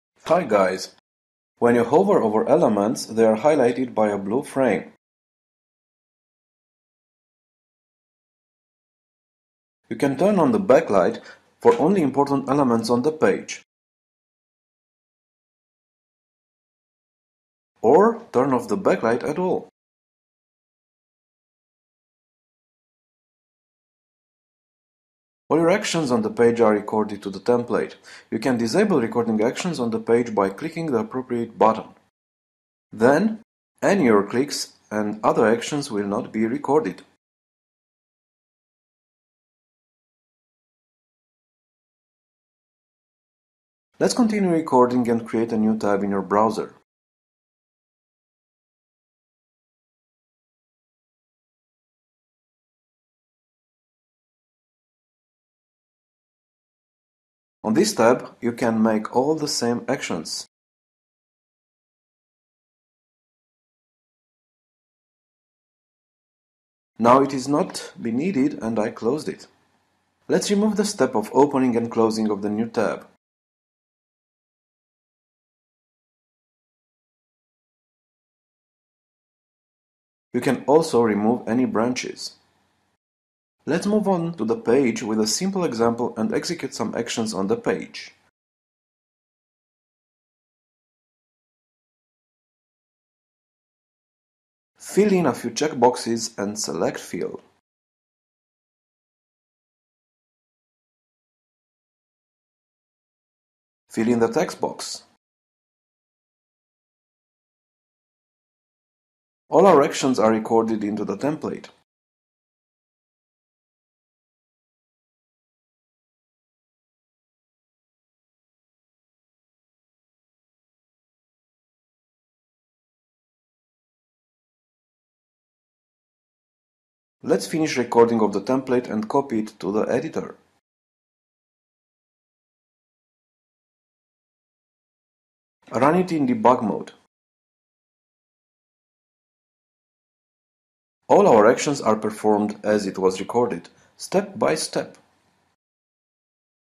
Basic video course: Get started (continuing)
This video shows how to record an easy template and execute it in debug mode.